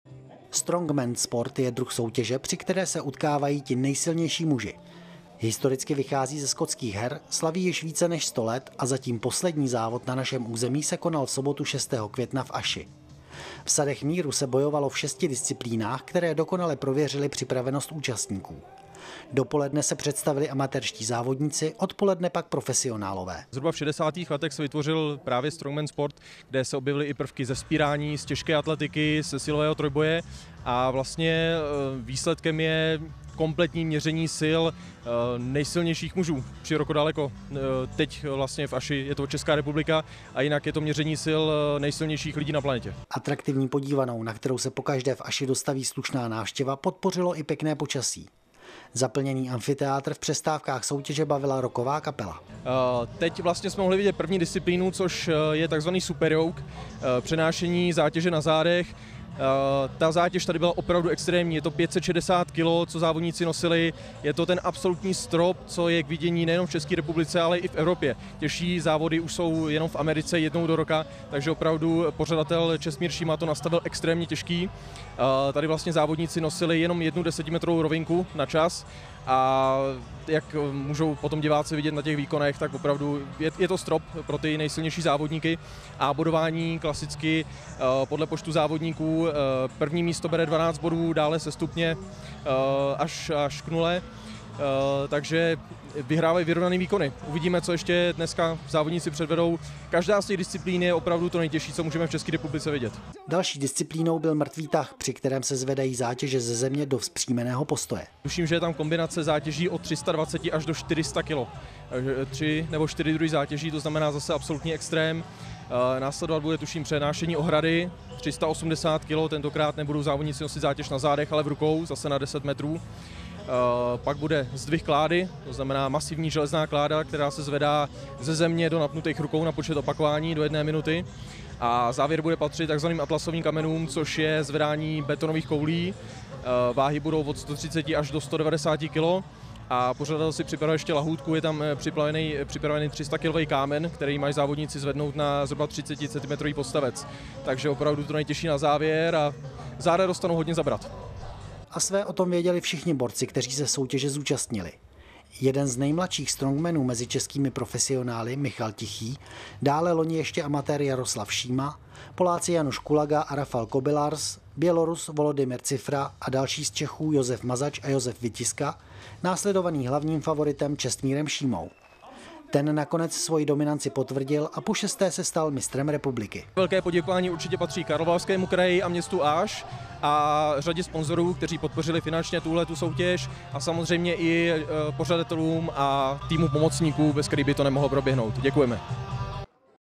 Aš: Strongmanem se stal opět Čestmír Šíma (TV Západ)
Strongman sport je druh soutěže, při které se utkávají ti nejsilnější muži. Historicky vychází ze skotských her, slaví již více než sto let, a zatím poslední závod na našem území se konal v sobotu 6. května v Aši. V Sadech Míru se bojovalo v šesti disciplínách, které dokonale prověřily připravenost účastníků. Dopoledne se představili amatérští závodníci, odpoledne pak profesionálové.

„Zhruba v šedesátých letech se vytvořil právě Strongman sport. Kde se objevily i prvky ze vzpírání, těžké atletiky a ze silového trojboje. Výsledkem je kompletní měření sil nejsilnějších mužů široko daleko. V Aši je to teď Česká republika. Jinak je to měření sil nejsilnějších lidí na planetě,“ doplnil Michal Širůčka, předseda výkonného výboru Českého svazu silových sportů.

Atraktivní podívanou, na kterou se pokaždé v Aši dostaví slušná návštěva, podpořilo i pěkné počasí. Zaplněný amfiteátr v přestávkách soutěže bavila rocková kapela.

„Teď jsme mohli vidět první disciplínu. Což je tzv. Super Yoke - přenášení zátěže na zádech. Ta zátěž tady byla opravdu extrémní. Je to 560 kg, co závodníci nosili. Je to ten absolutní strop, co je k vidění nejenom v České republice, ale i v Evropě. Těžší závody jsou jednou do roka už jen v Americe. Takže opravdu pořadatel Čestmír Šíma to nastavil extrémně těžké. Tady vlastně závodníci nosili jenom jednu desetimetrovou rovinku na čas a jak mohou diváci poté vidět na těch výkonech, tak opravdu je to strop i pro ty nejsilnější závodníky. Bodování je klasicky podle počtu závodníků – 1. místo bere 12 bodů. Dále je to sestupně až k nule. Takže vyhrávají vyrovnané výkony. Uvidíme, co ještě dneska závodníci předvedou. Každá z těch disciplín je opravdu to nejtěžší, co můžeme v České republice vidět,“ řekl Širůčka.

Další disciplínou byl mrtvý tah, při kterém se zvedají zátěže ze země do vzpřímeného postoje.

„Tuším, že je tam kombinace zátěží od 320 až do 400 kg. Tři nebo čtyři druhy zátěží, tzn. zase absolutní extrém. Následovat bude tuším přenášení ohrady, 380 kg tentokrát nebudou závodníci nosit jako zátěž na zádech, ale v rukou, zase na deset metrů. Pak bude zdvih klády. Masivní železná kláda se zvedá ze země do napnutých rukou na počet opakování do jedné minuty. Závěr bude patřit tvz. Atlasovým kamenům. Což je zvedání betonových koulí. Váhy budou od 130 až do 190 kg. Pořadatel si připravil ještě lahůdku. Je tam připravený 300 kg kámen, který mají závodníci zvednout na zhruba 30 cm podstavec. Takže opravdu to nejtěžší na závěr. Záda dostanou hodně zabrat,“ dodal Širůčka.

A své o tom věděli všichni borci, kteří se soutěže zúčastnili. Jeden z nejmladších strongmanů mezi českými profesionály Michal Tichý, dále loni ještě amatér Jaroslav Šíma, Poláci Janusz Kulaga a Rafal Kobylarz , Bělorus Volodymyr Cyfra, další z Čechů Josef Mazač a Jaroslav Vytiska následovaný hlavním favoritem Čestmírem Šímou. Ten nakonec svoji dominanci potvrdil a pošesté se stal Mistrem republiky.

„Velké poděkování určitě patří Karlovarskému kraji, městu Aš a řadě sponzorů, kteří podpořili finančně tuto soutěž. Samozřejmě i pořadatelům a týmu pomocníků, bez kterých by to nemohlo proběhnout. Děkujeme,“ vzpomněl na pomoc s organizací Širůčka.